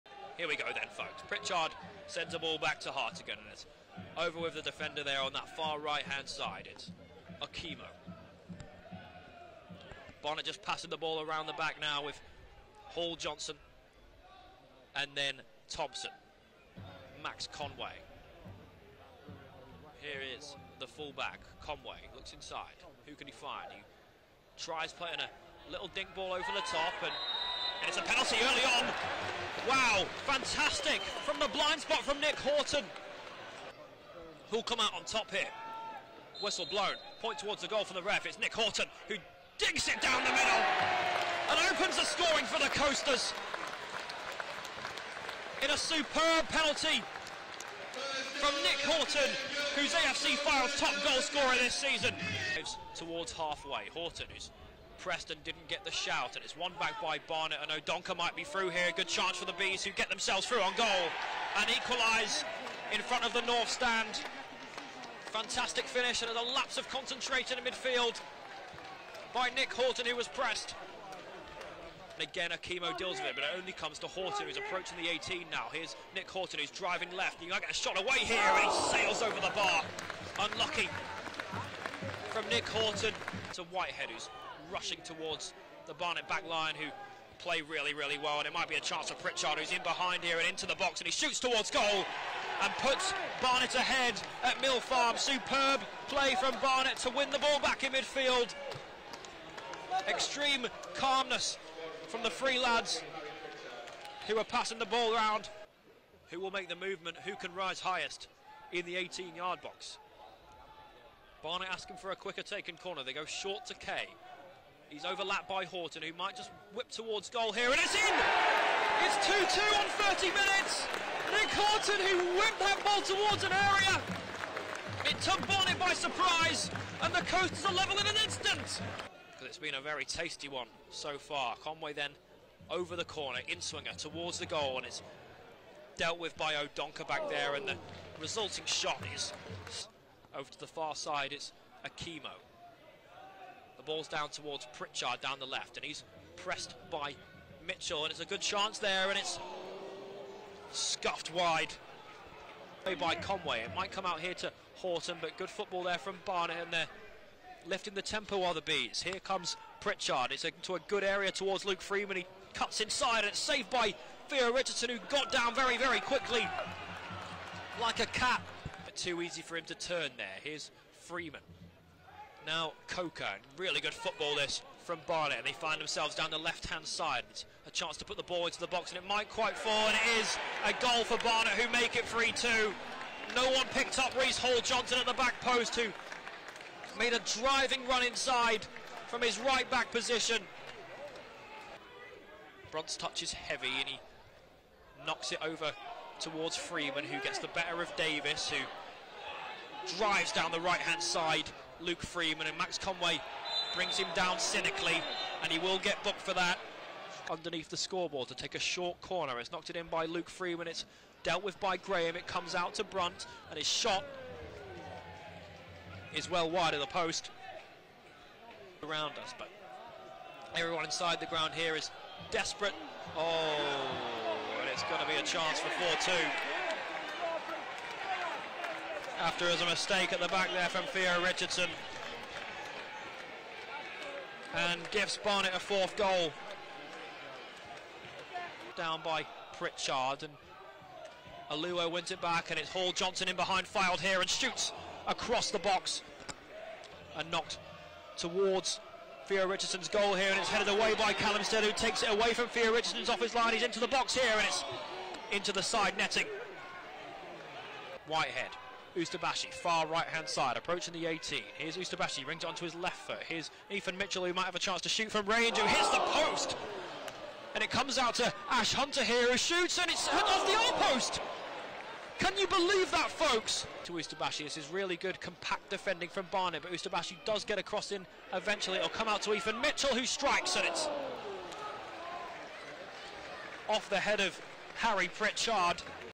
Match Highlights | AFC Fylde 2-4 Barnet
The highlights from The Coasters' frustrating afternoon at Mill Farm as our seven-game unbeaten run came to an end to the hand of The Bees who came out 4-2 victors on the Fylde Coast.